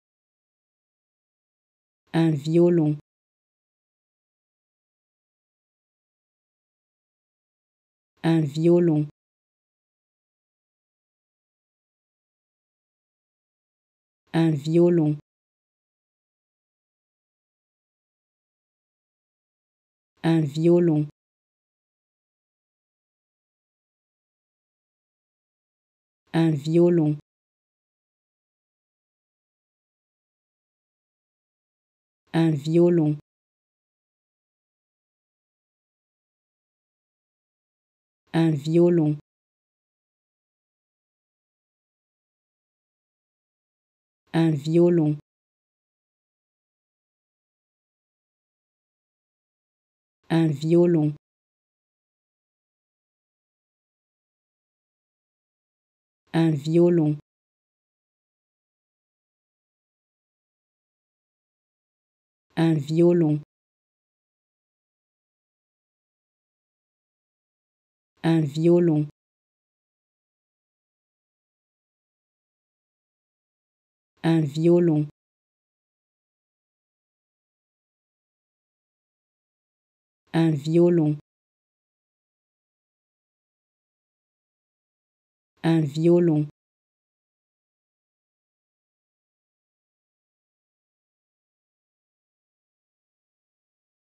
How to Pronounce ''Un violon'' (A violin) Correctly in French
Learn how to say and properly pronounce ''Un violon'' in French with this free pronunciation tutorial.